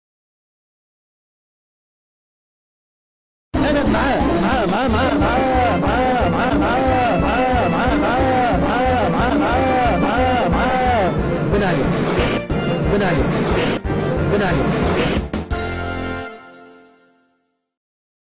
Jaafar Guesmi chante avec le dictateur Ben Ali!
Jaafar Guesmi